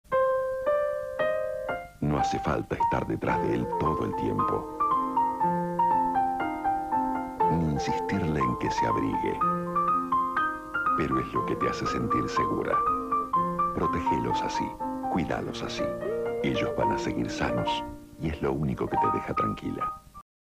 Publicidad Agua Lavandina Sello Rojo Uruguay II (2001)
Segunda publicidad de la reconocida lavandina emitida en los canales 4, 10 y 12 en 2001.